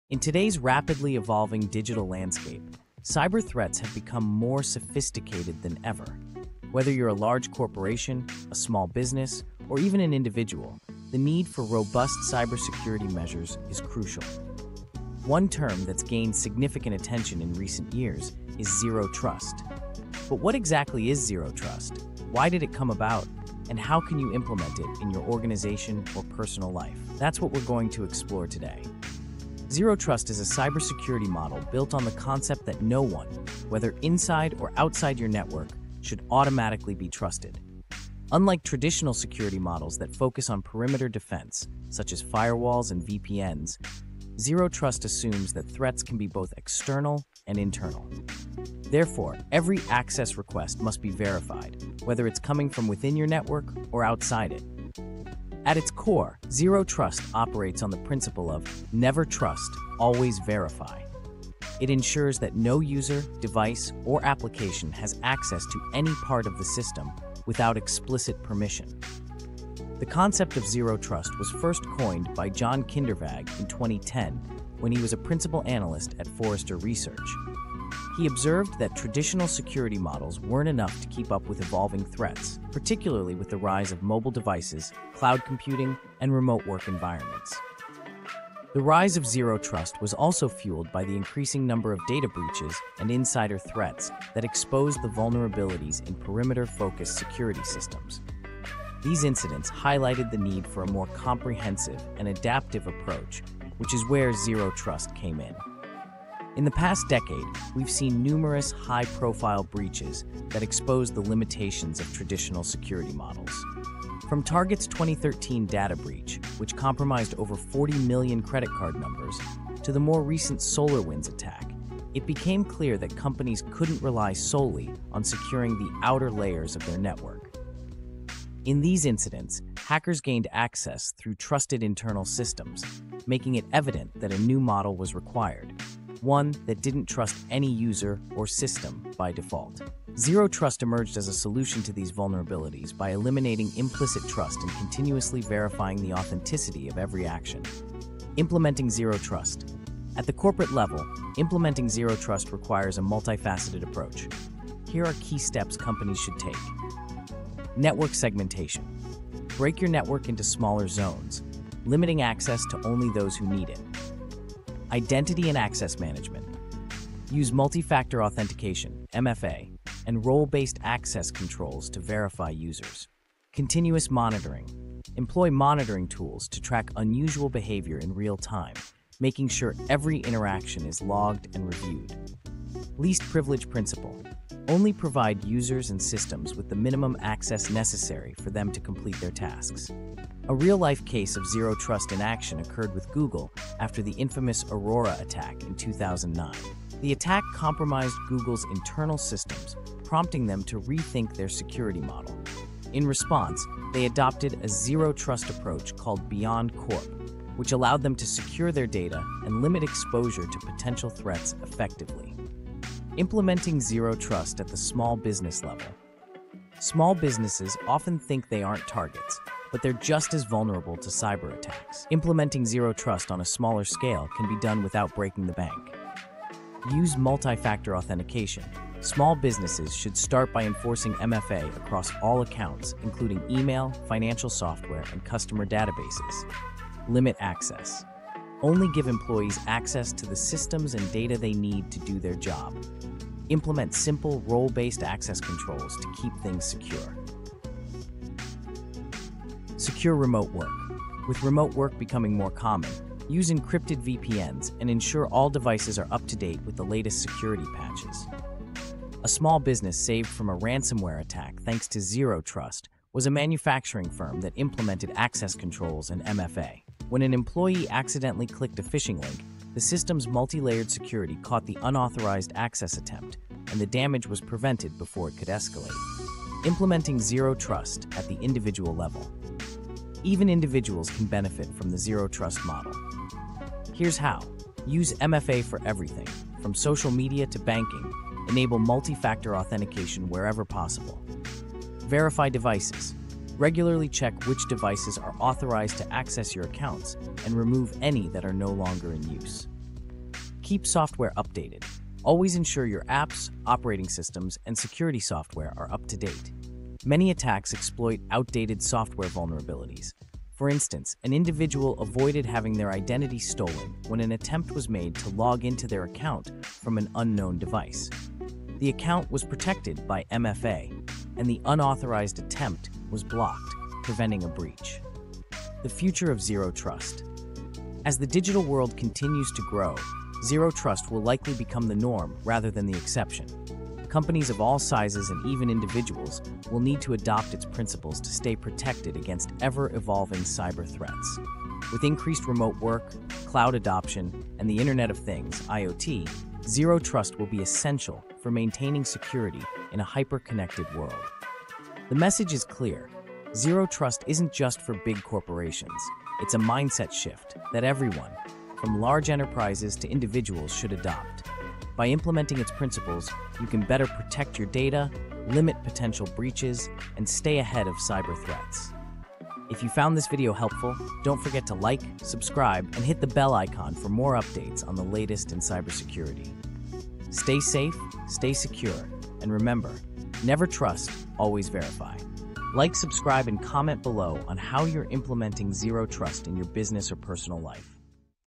What is Zero Trust?
In this video, we explore the concept of Zero Trust, a revolutionary cybersecurity model that can protect businesses and individuals from modern digital threats. Discover what Zero Trust is, its history, and why it came about, along with real-world examples where Zero Trust prevented or mitigated cyberattacks. We also provide practical steps on how to implement Zero Trust, whether you’re managing a corporation, a small business, or securing your personal data.

🔒 Topics Covered:

Understanding Zero Trust: What is it and why does it matter?
A brief history of Zero Trust and its evolution in cybersecurity
How corporations, small businesses, and individuals can implement Zero Trust

Real-world case studies showcasing Zero Trust in action
Practical cybersecurity tips for enhancing digital safety
For more information about our services and how we can help you implement Zero Trust in your organization, contact Stratigos Dynamics, Inc. (SDI):